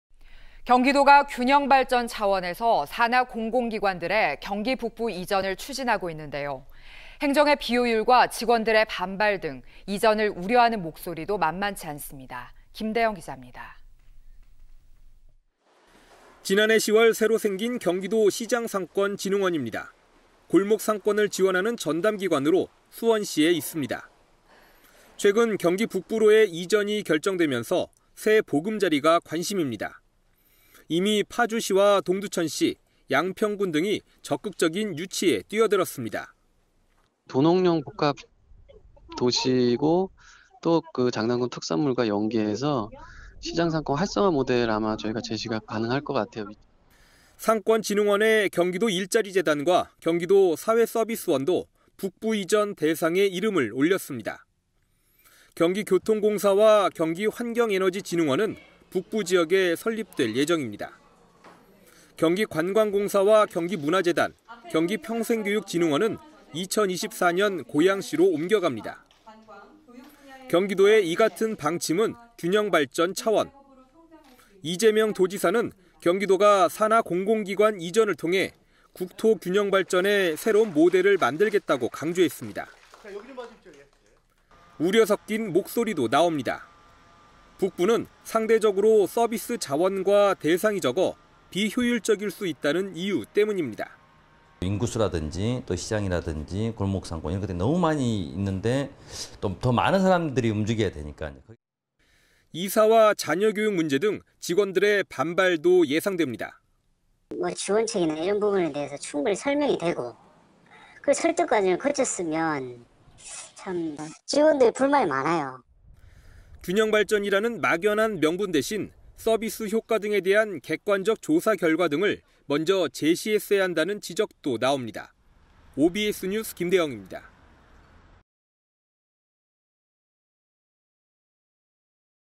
경기도 공공기관 북부 이전 '동상이몽'
경기도가 균형발전 차원에서 산하 공공기관들의 경기북부 이전을 추진하고 있는데요.
행정의 비효율과 직원들의 반발 등 이전을 우려하는 목소리도 만만치 않습니다.
김대영 기자입니다.